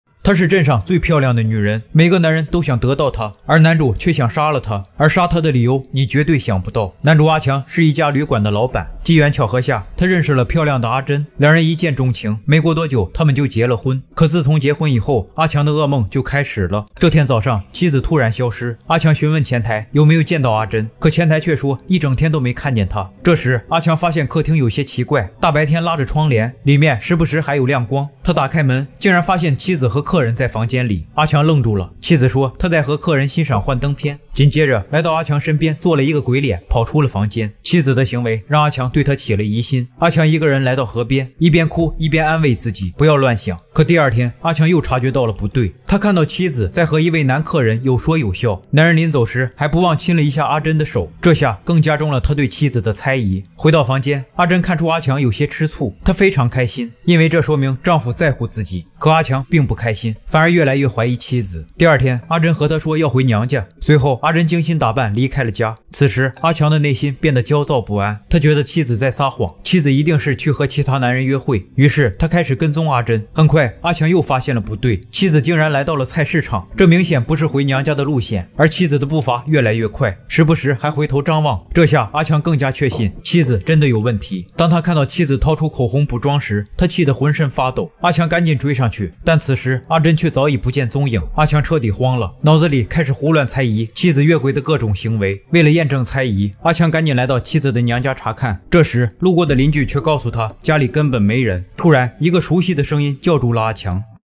妻子長得太美!丈夫每天提心吊膽,生怕失去妻子!
No.2 | 女孩坐飛機遭空姐離奇要求，驚現空難電影情節！真實故事改編震撼人心